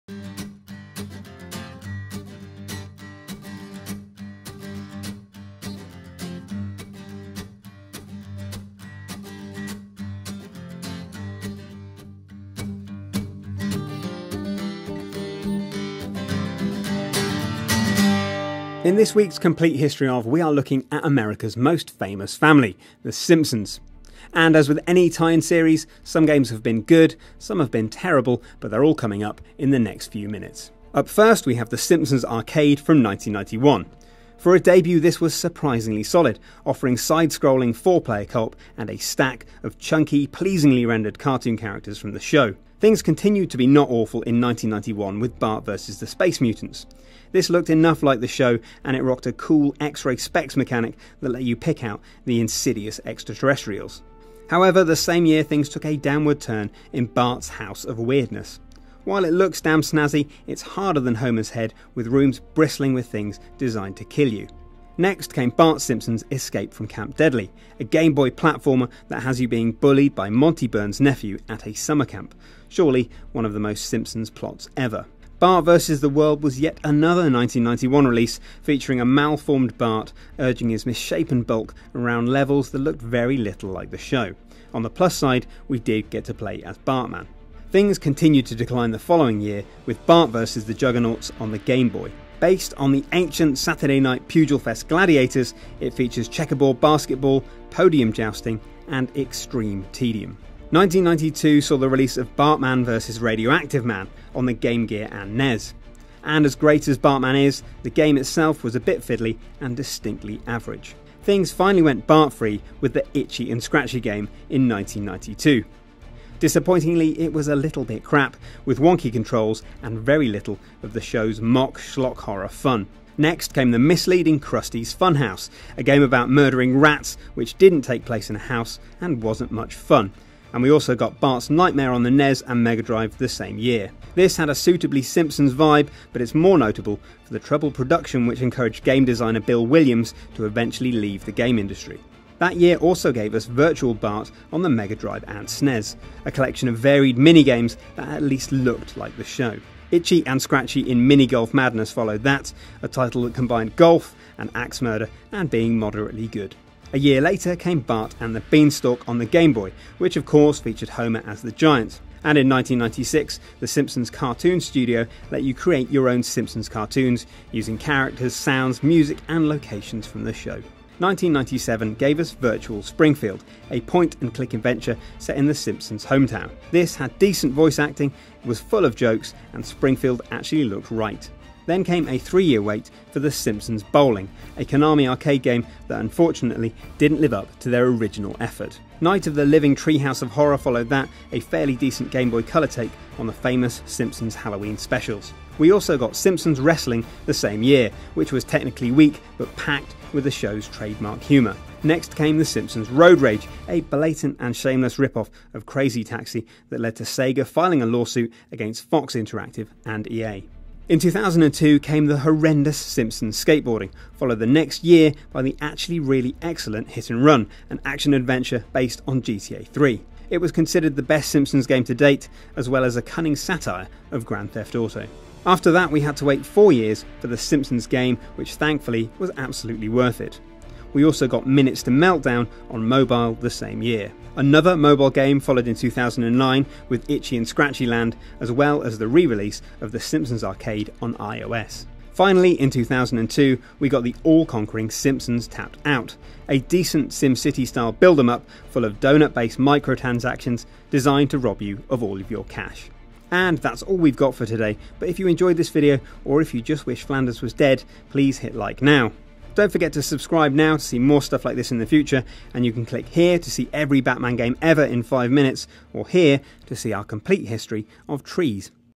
A Complete History of The Simpsons in Games
Welcome to our new weekly show, A Complete History of...

This week Matt talks us through every video game appearance of America's most famous family, The Simpsons. Some good, some bad, all here.

And don't forget to tell us about your favourite Simpsons memories in the comments, and yet us know which game was you remember most fondly.